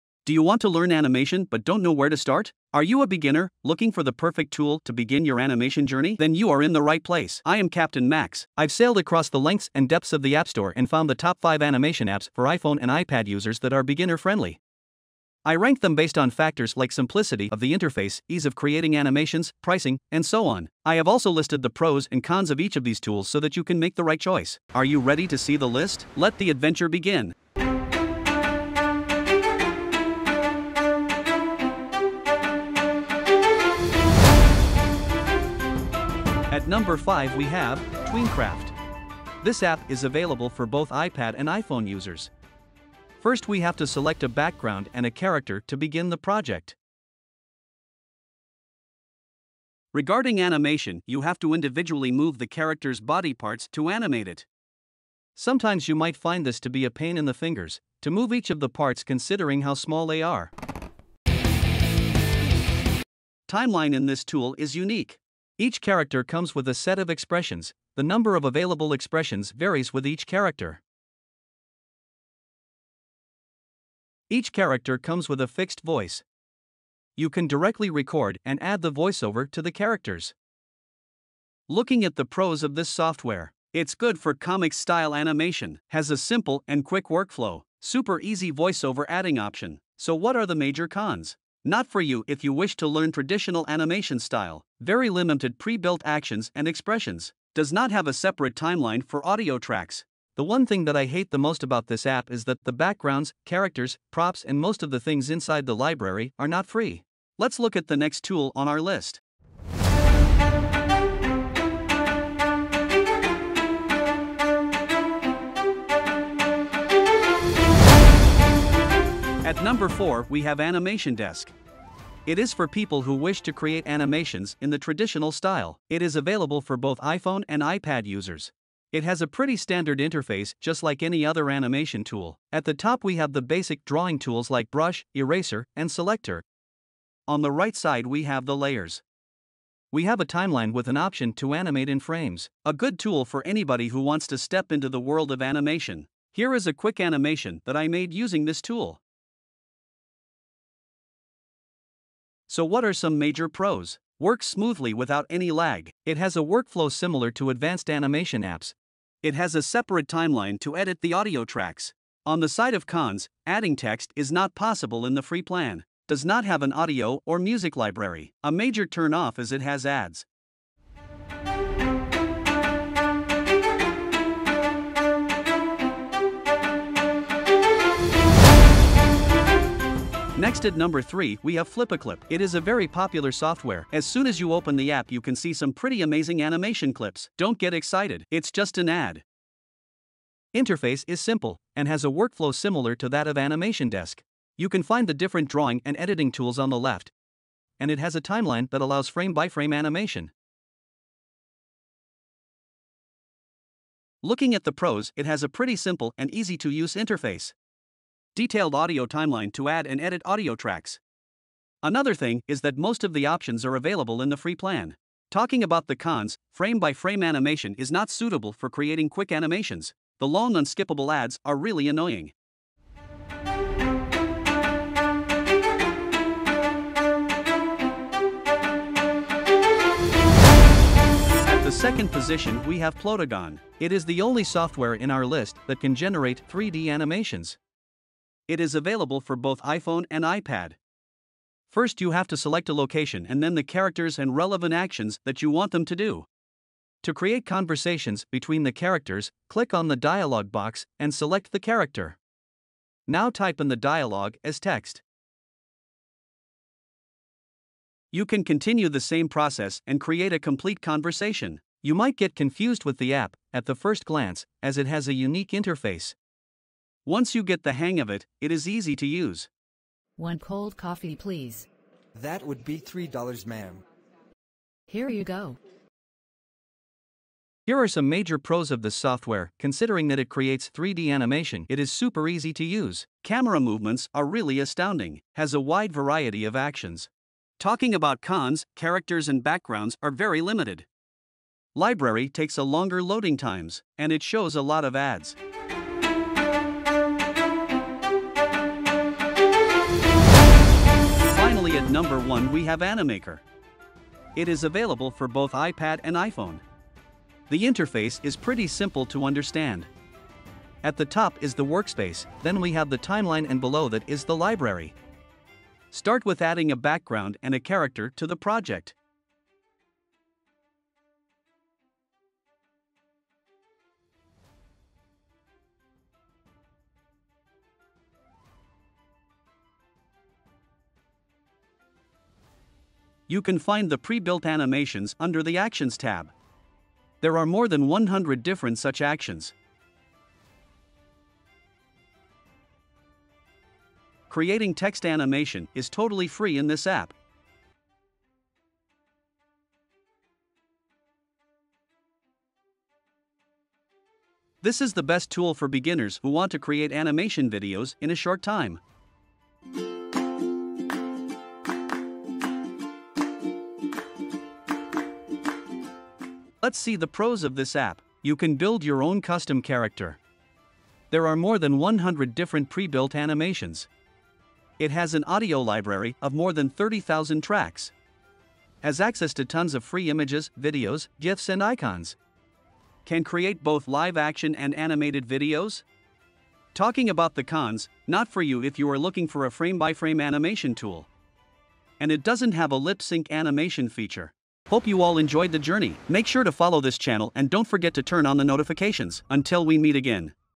Top 5 Best animation apps for iPhone and iPad
We have listed down the top 5 best animation apps on ipad and iphone that are beginner friendly. There are hundreds of animation apps on the appstore. However only a few are beginner friendly. We went through the arduous task of going over all the apps and created this list.

Timestamps:
Best animation apps - 0:00
Tool number 5 (Tweencraft) - 0:35
Tool number 4 (Animation Desk) - 2:00
Tool number 3 (Flipaclip) - 3:00
Tool number 2 (Plotagon) - 3:50
Tool number 1 (Animaker) - 5:00
Conclusion - 6:35